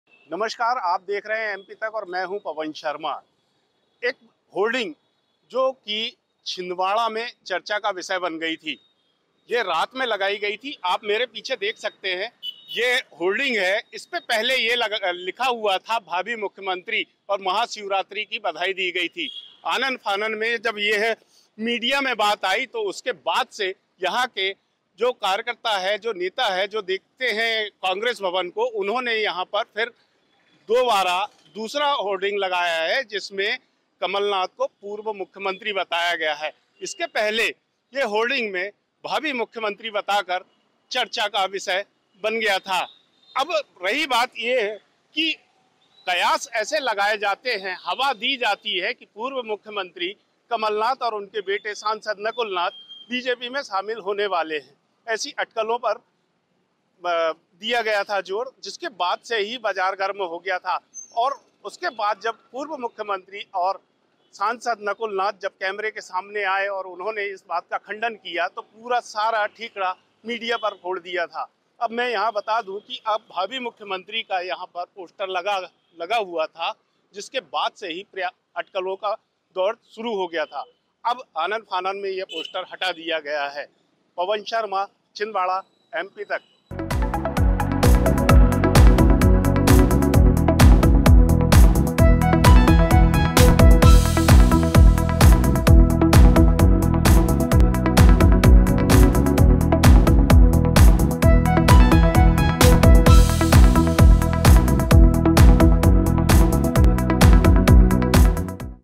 Chhindwara News: Kamal Nath को लेकर फिर हलचल तेज, लग गए पोस्टर, लिखा गया भावी मुख्यमंत्री !|MP Tak
Madhya Pradesh विधानसभा चुनाव से पहले कांग्रेस कार्यकर्ता Kamal Nath  के नाम के बाद भावी मुख्यमंत्री लिखते थे। विधानसभा चुनाव में कांग्रेस की हार हुई है। इसके बावजूद छिंदवाड़ा में कांग्रेस के लोग कमलनाथ को भावी मुख्यमंत्री ही बता रहे हैं. होर्डिंग में लगे पोस्टर वायरल होने के बाद कांग्रेस कार्यकर्ताओं ने पोस्टर बदल कर कमसनाथ के नाम के आगे पूर्व मुख्यमंत्री लिखा...देखिए पूरी रिपोर्ट